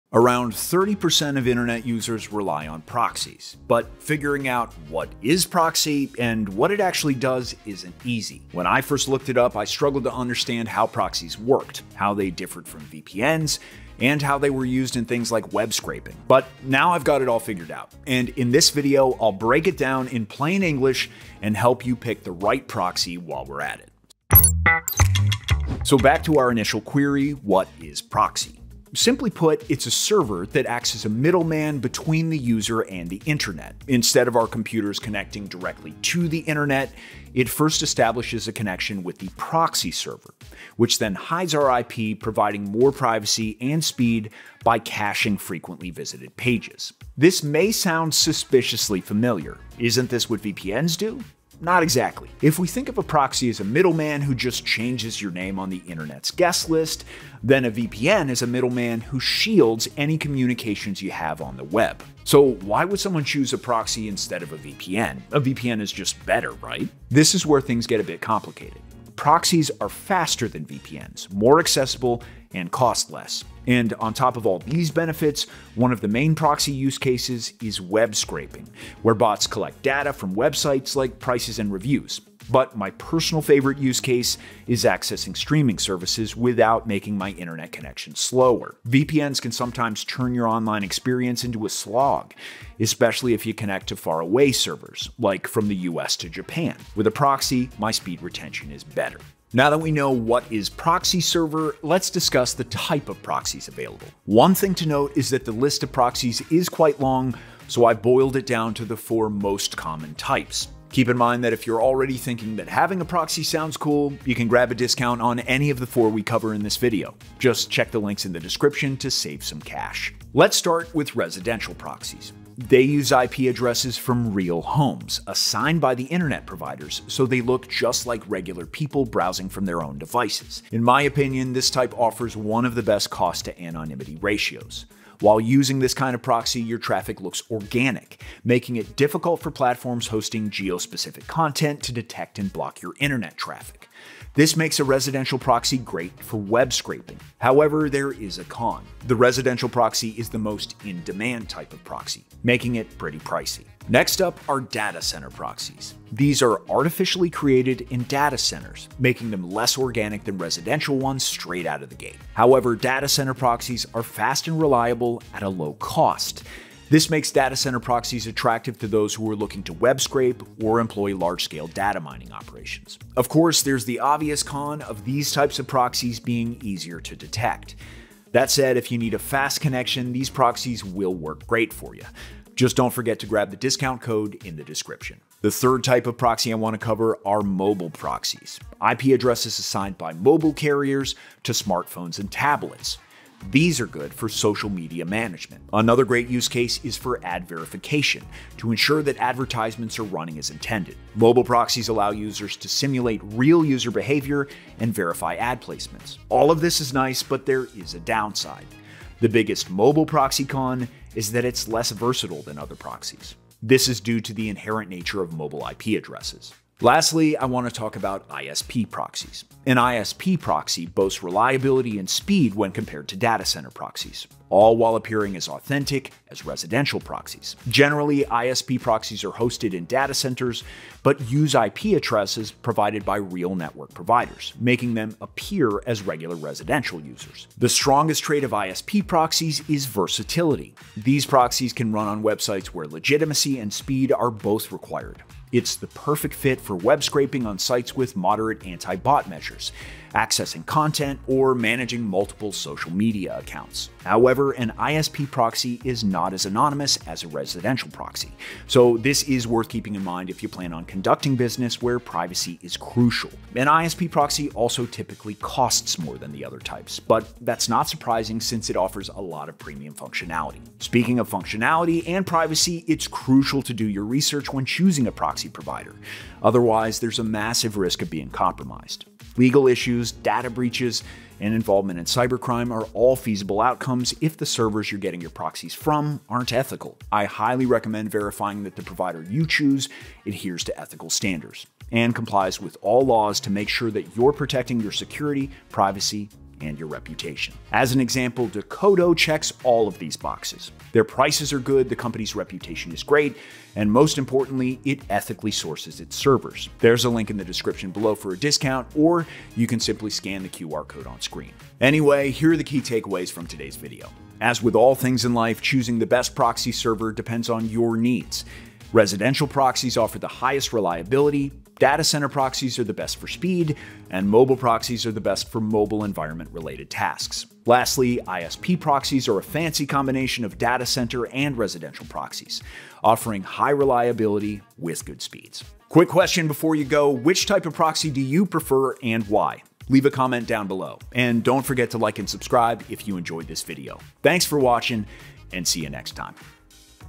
What is a proxy? ft. Decodo | Proxy types explained in 2026
✨ Get the best discount for Decodo here and save big cash!✨

In this proxy explanation video, I explore the main types of proxies and explain their use cases.

00:00 Intro
00:25 What is a proxy?
01:40 Main types of proxies - Residential proxy
02:34 Datacenter proxy
03:05 Mobile proxy advantages
03:37 What is ISP proxy?
05:12 Conclusions